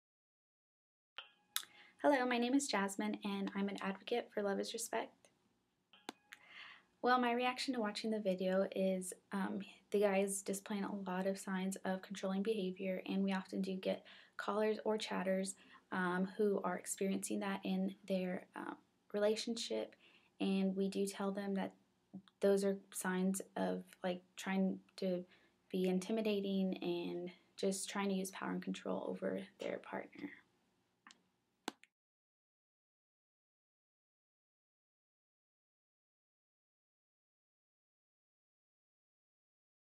Jasmine Talks About Controlling Via Webcam
Our peer advocate Jasmine talks about Adam's controlling behavior shown via webcam to his girlfriend back home. (Real World episode "Multiple Headaches")